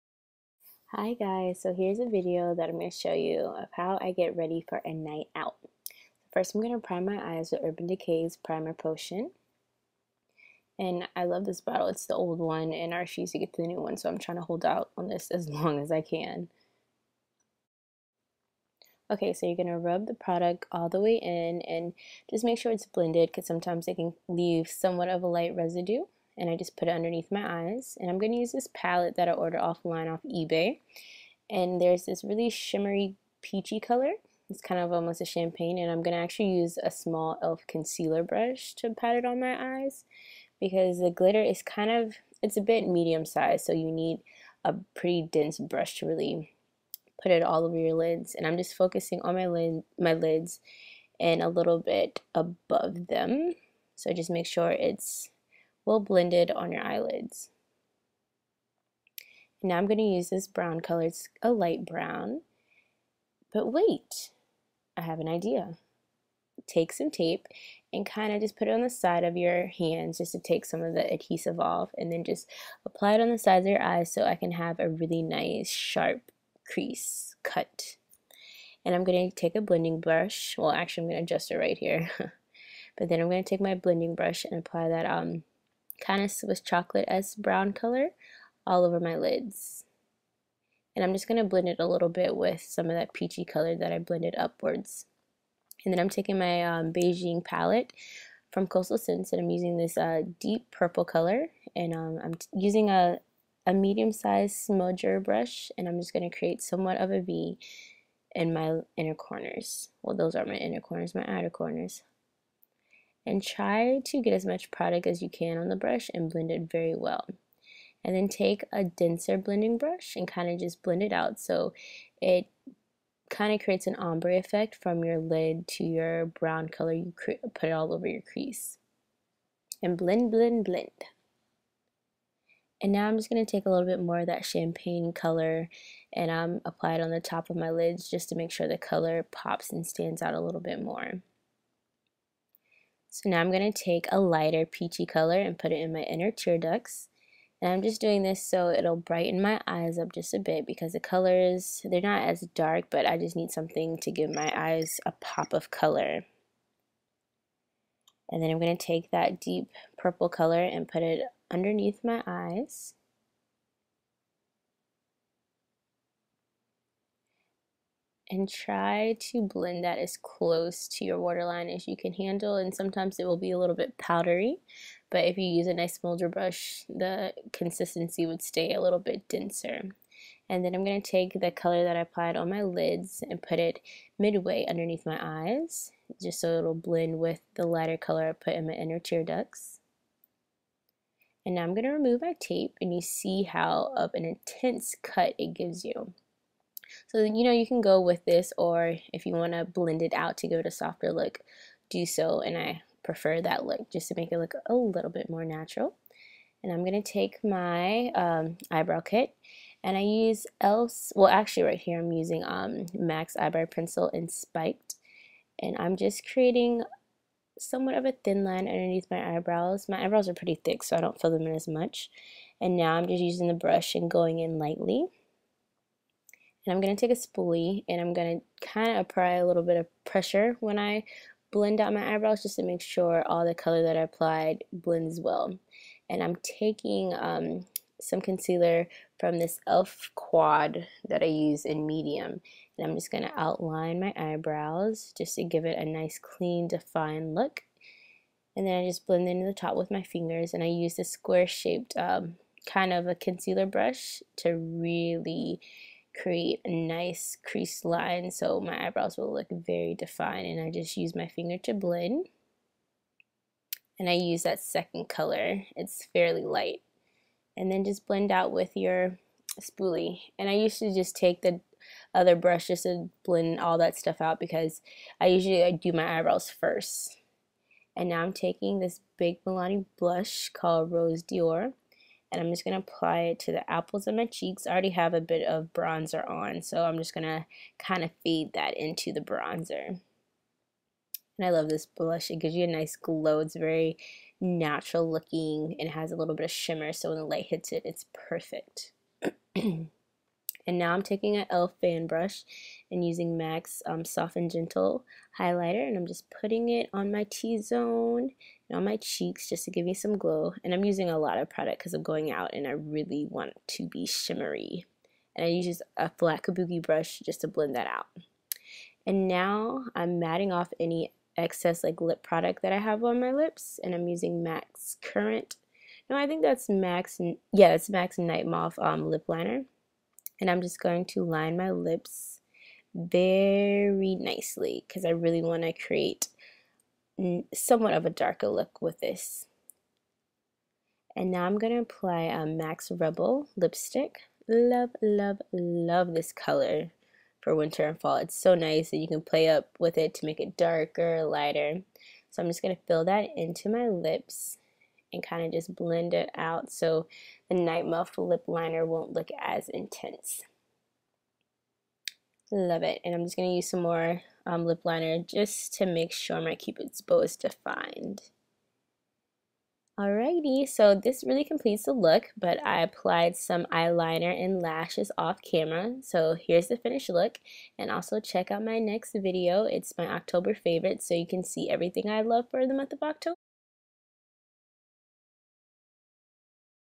Tutorial | Easy Vampy Night Out
Easy Vampy Look for a Night Out!

Costal Scents Beijing Palette
Mac Spiked Eyebrow Pencil
e.l.f. medium concealer kit
Rose D' Oro Milani Baked Blush
Mac Soft and Gentle
Mac Night mouth Lip Liner
Mac Rebel